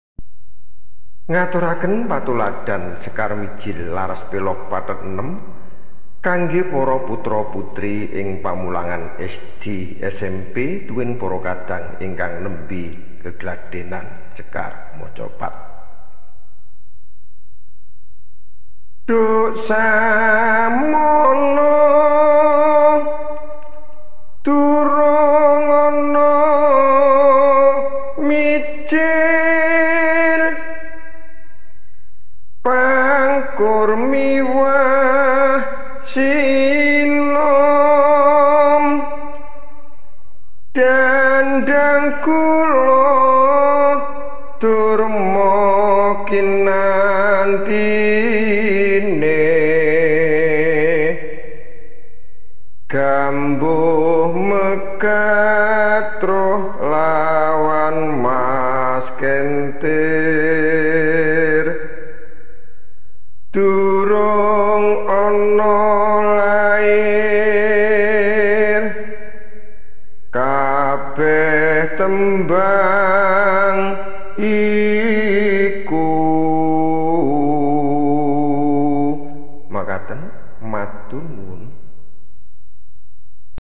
TUTORIAL NEMBANG MACAPAT MIJIL LARAS PELOG Pt 6 Oleh Puguh W
Tutorial Pembelajaran Nembang Macapat untuk anak SD/SMP dan Pemula
Sekar Mijil Laras Pelog Pathet Nem @puguhwidodo5287